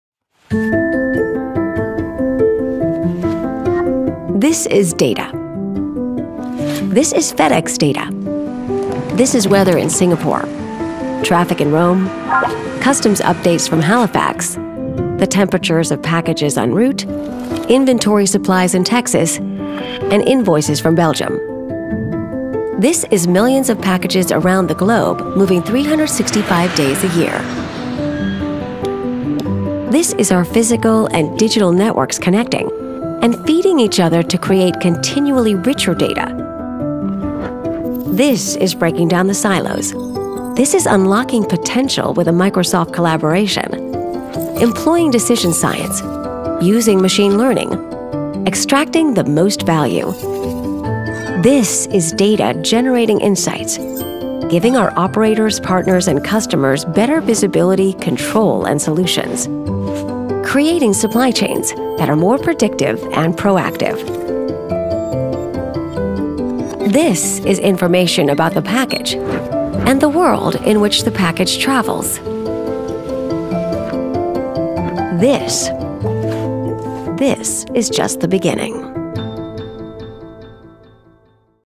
FedEx Data Transformation
We’re transforming how we use technology and data about customer packages to drive value for our business and our customers.